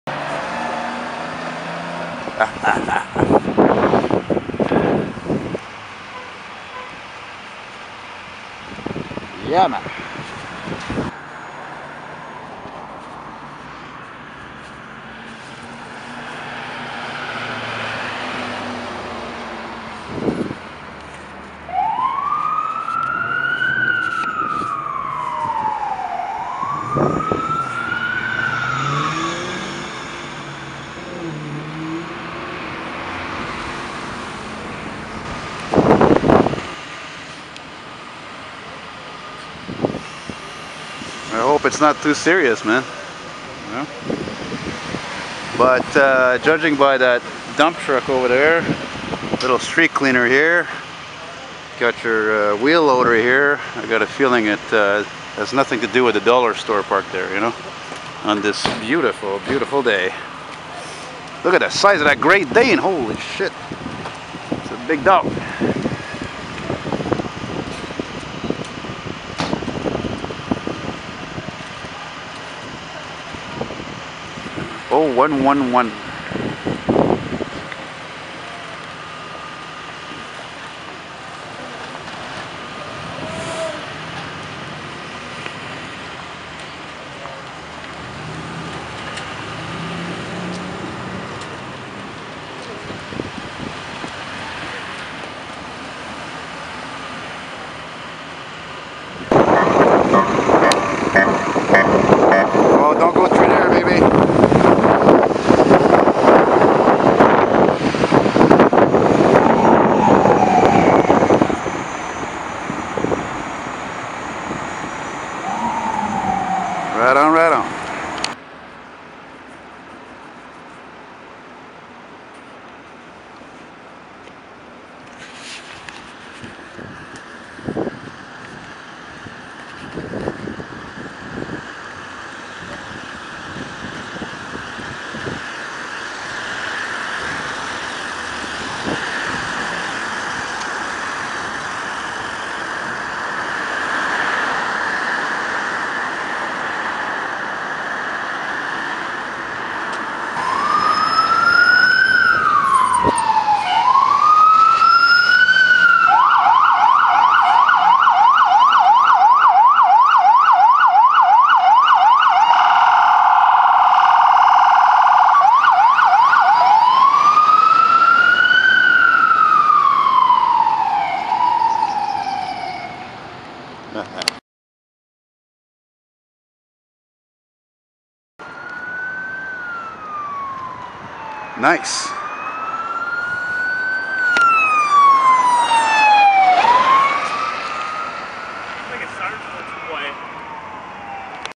URGENCES SANTE AMBULANCES RESPONDING IN MONTREAL
SEEN DOWNTOWN, ANJOU, HOCHELAGA AND IN ST HENRI.